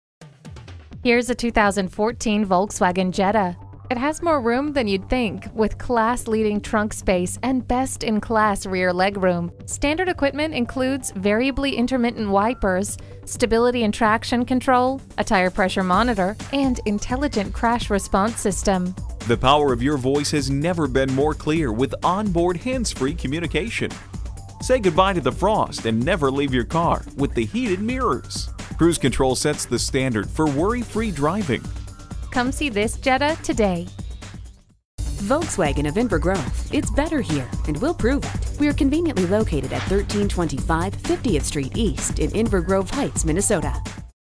2014 Volkswagen Jetta Sedan St-Paul MN Minneapolis, MN #70076 - SOLD
Year: 2014
Make: Volkswagen
Model: Jetta Sedan
Trim: 4dr Auto SE w/Connectivity PZEV
Engine: 1.8 L 4 Cyl. Cyl.
Transmission: Automatic
Color: Black Uni
Stock #: 70076

Address:
1325 50th Street East